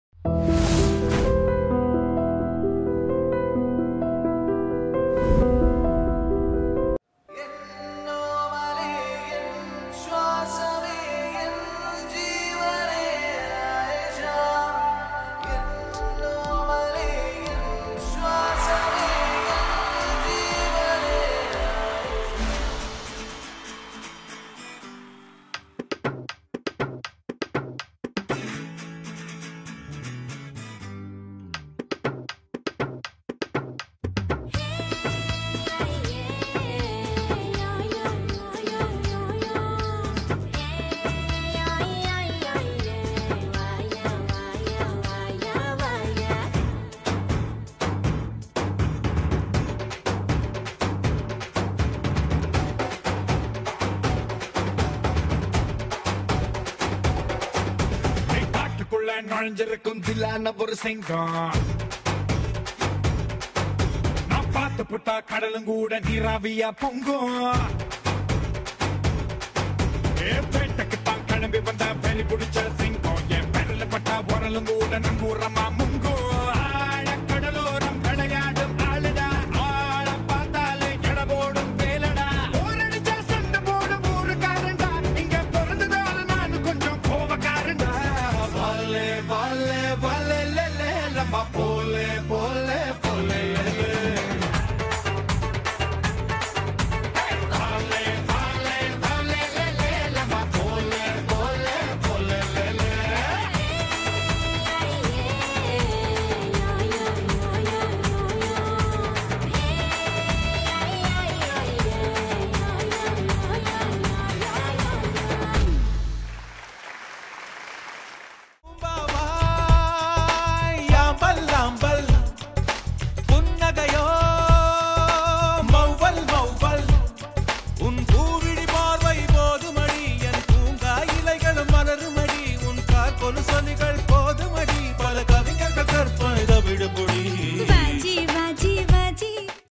Isha Talwar Exclusive Dance Performance | SIIMA 2014
Watch Actress Isha Talwar Exclusive Dance Performance at SIIMA 2014 Awards FUnction, Malaysia.